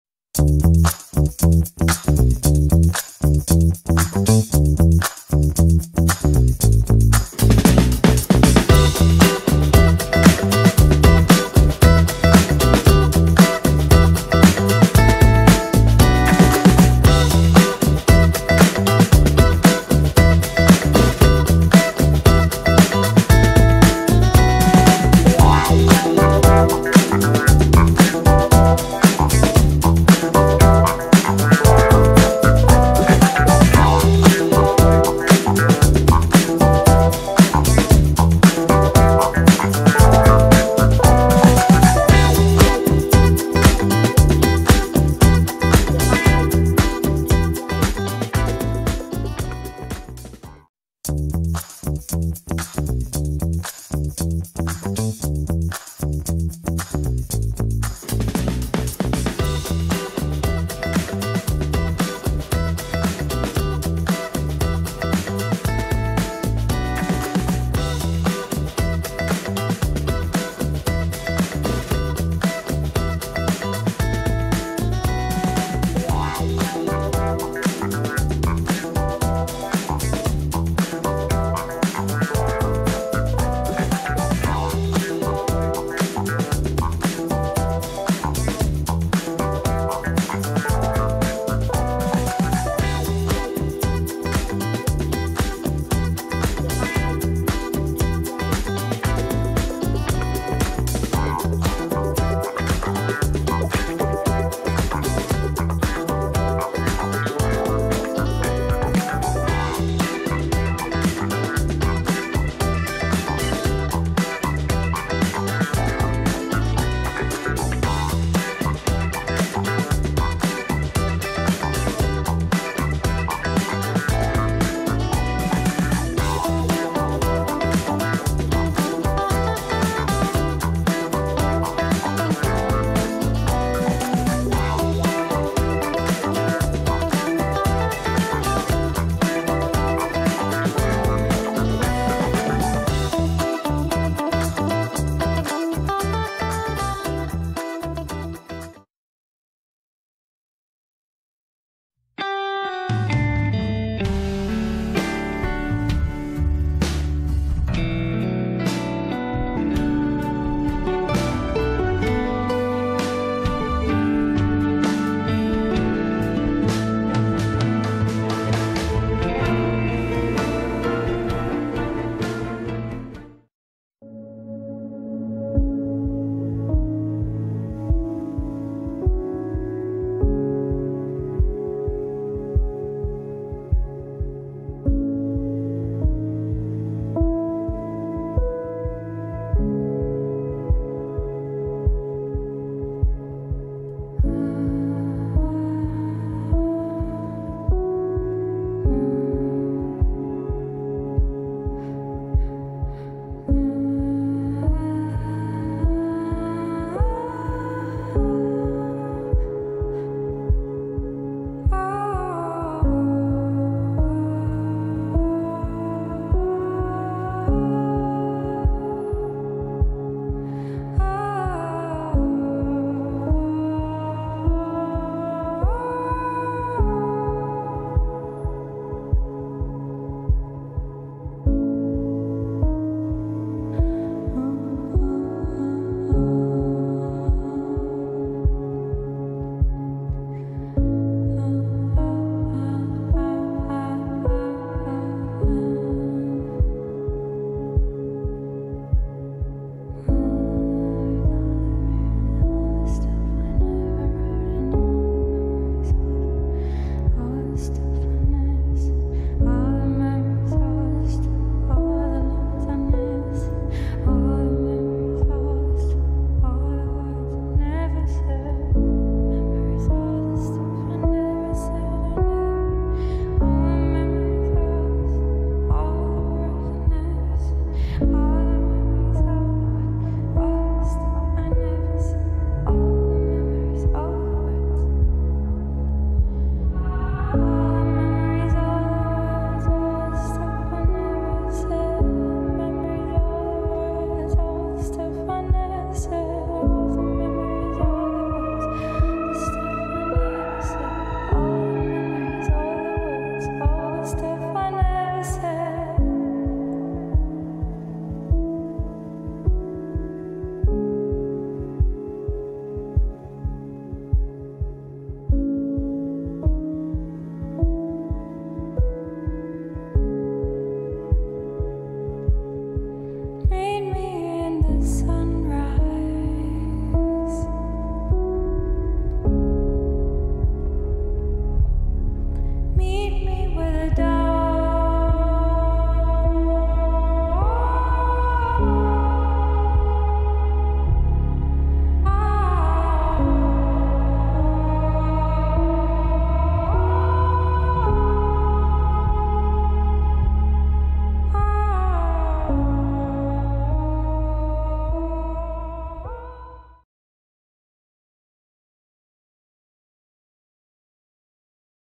Family History Project Magazine April 2022
Birthdays, Anniversaries - NEW feature - Birthdays In Memorium,  Honoring our family born 1890 and later and died before 2023. If We've missed someone PLEASE let us know..